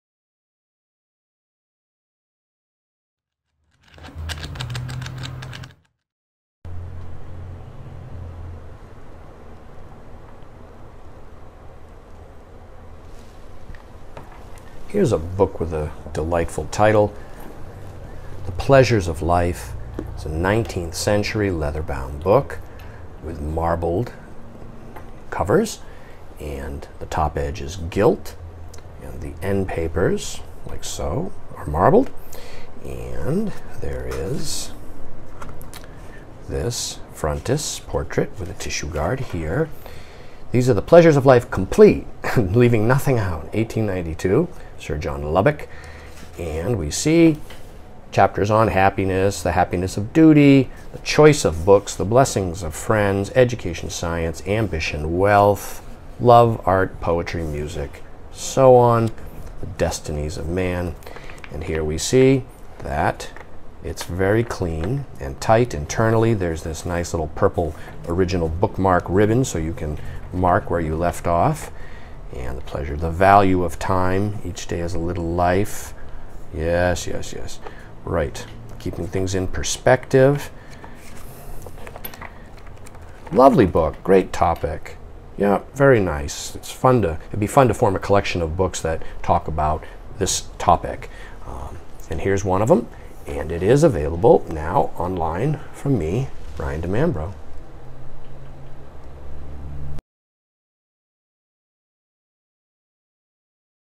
Life Pleasures 1892 Sir Lubbock gift book leather Love Wealth Value of Time Art Troubles Poetry
[Success in Life, Making the most of Life, Life Philosophy in Victorian England, late 19th century British cultural history, positive thinking].

The Pleasures of Life, Complete by the Right Hon. Sir John Lubbock, Bart. M.P.
Published 1892, Philadelphia, by Henry Altemus. 332 pages.

Attractive grained morocco 19th century 1/2 leather binding. Raised bands, gilt title, covers marbled paper. Marbled end papers top edge gilt. Photo-offset frontis portrait with tissue guard. Slightly deckled edges, fine typography and hand-made paper throughout.

Lovely and well produced late 19th century small pocket sized personal gift book.

A delightful small compilation of essays and speeches given by Sir John Lubbock, a leading Victorian banker and politician who was instrumental in establishing the science of Archaeology. As a member of Parliament, he introduced laws to protect Britain's archaeological heritage. This book offers a glimpse into his own personal history, and the values he held which to this day resonate with insight and inspirational reason.

A wonderful theme to form a collection of antiquarian books around.

Book is well preserved, overall clean, fresh and pleasing.

Book measures: 6 1/4" H X 4 1/2" W.

[V1732].